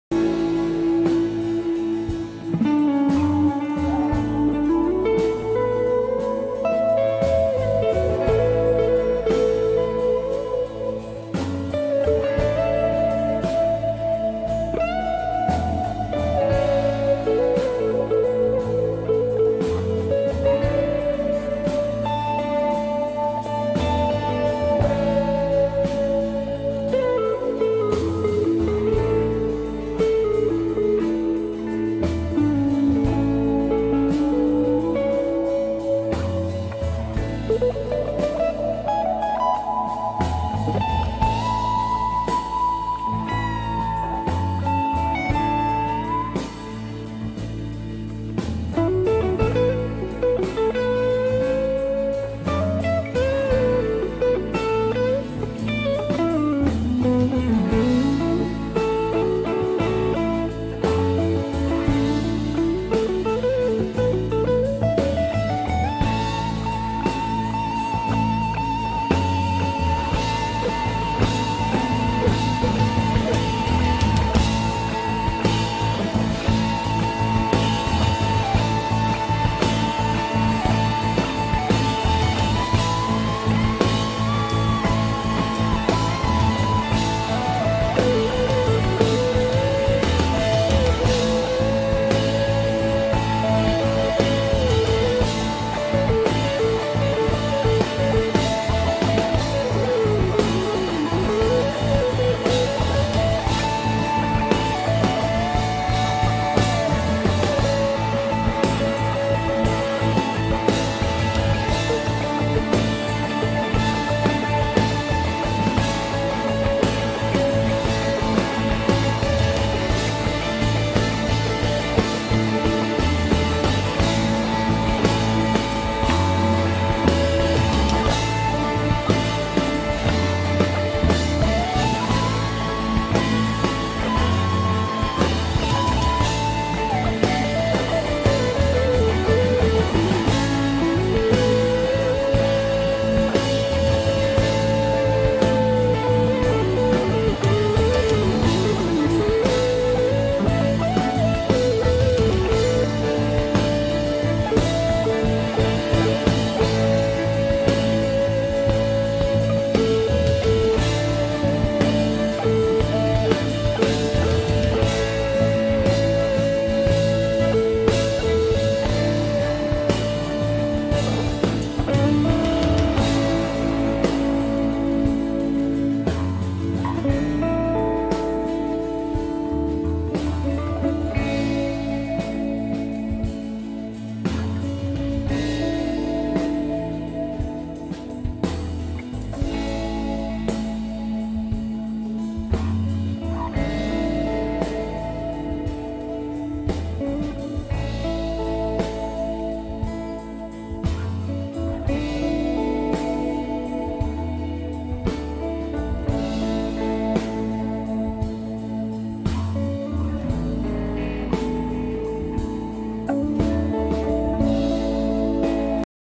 Jammin in the name of. fractal axe fx3, PRS se, bootstrap pickups alnico 3 neck sweet Serrano
this is the ODS model on the AxeFX 3, fractal 412 greenback, and vintage 30 impulse responses. the guitar is a Paul Reed Smith custom 22 SE semi hollow, that's been reloaded with bootstrap pickup suites Serrano humbucker set. the net pickup that is used in this solo is an alnico 3 magnet.